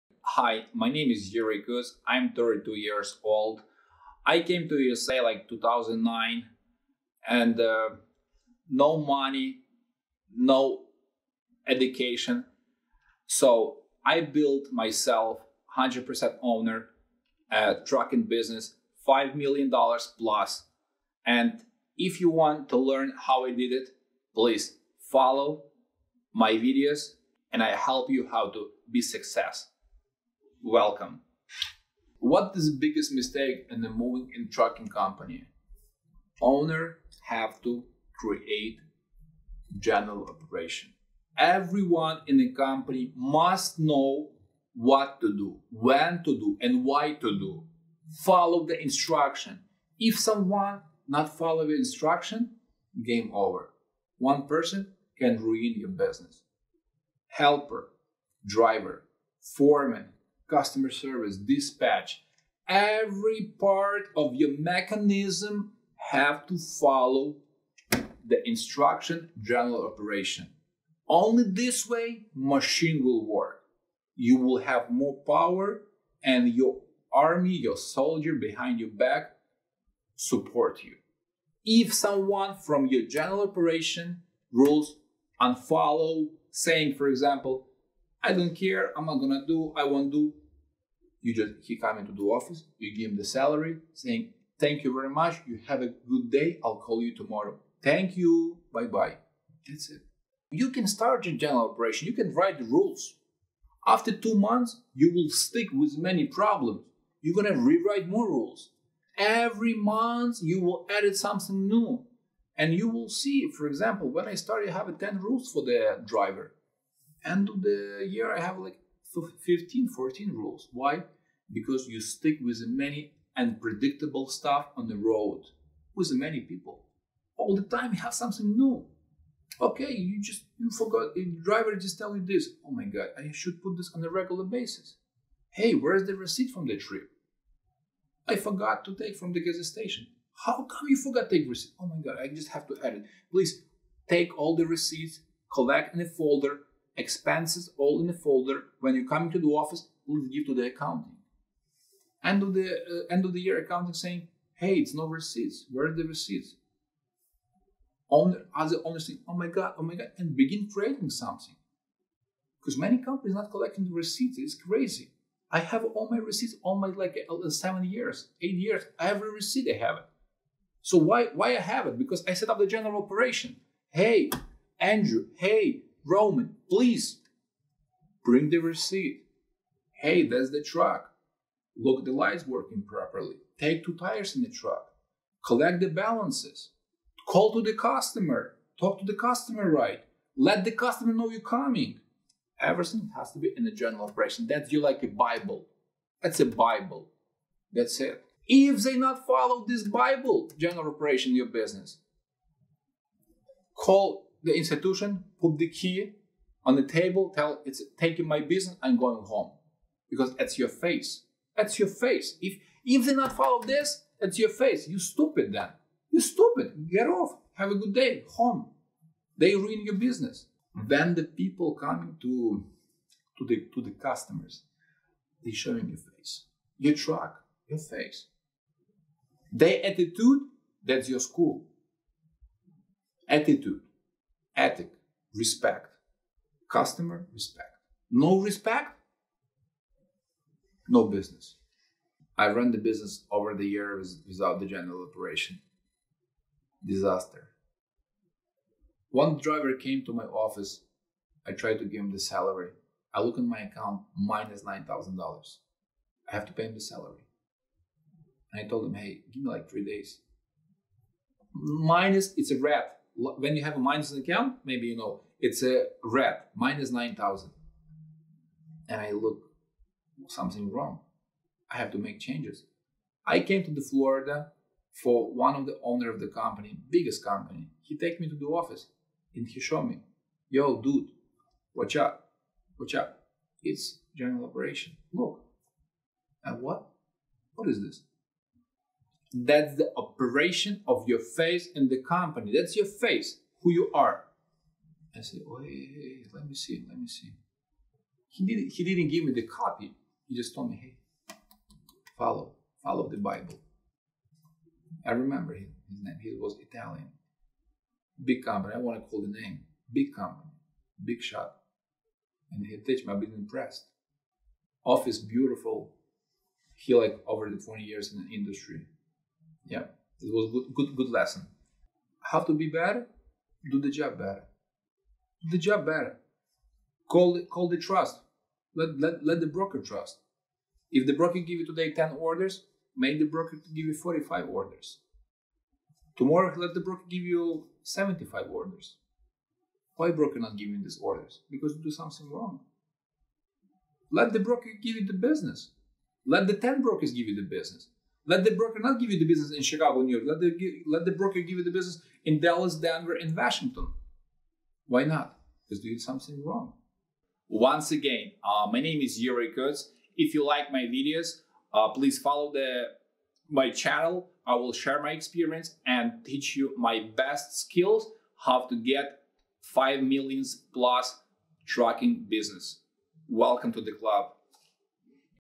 The Biggest Mistakes Business Owners Make - Trucking & Moving Business Tips
What are the biggest mistakes in the trucking and moving business? From my own experience:

- Owners need to create a general operation because everyone in the company must know what to do, when to do and why to do it at all times.
- Keep the system updated so there are no problems.

I ran my business without general operation for years, it was a disaster! I learned my lesson by listening to other more experienced business owners in the industry and now I only rely on the business bible (General Operations)